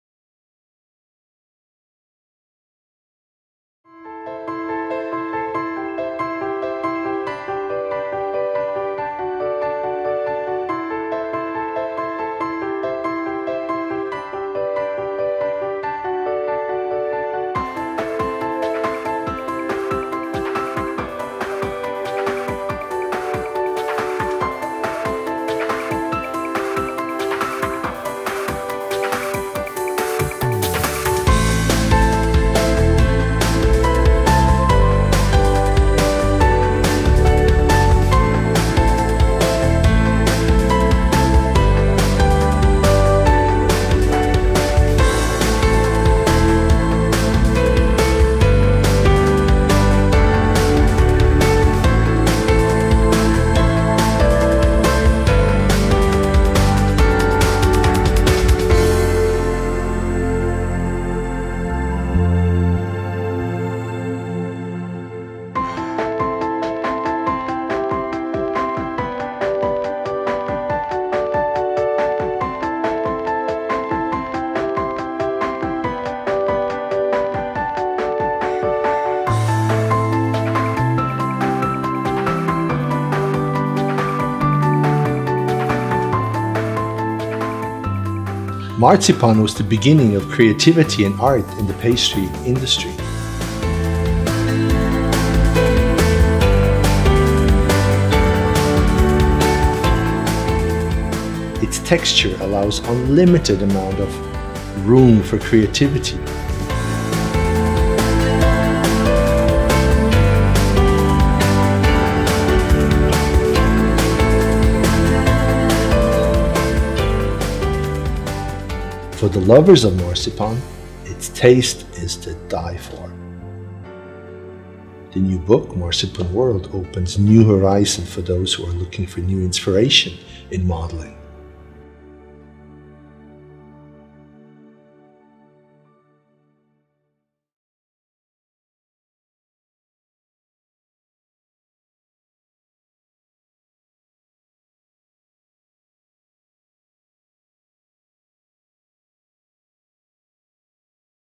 Trailer Marzipan World von Franz Ziegler
Eine besonders süße Neuigkeit: im September erscheint Marzipan World von Franz Ziegler. Einen ersten Einblick gibt es aber schon heute mit dem Trailer. Wir freuen uns schon sehr!

A specialy sweet treat: in September Franz Ziegler's new book "marzipan world" is being released. The trailer gives a sneak preview and a first look. We are very excited!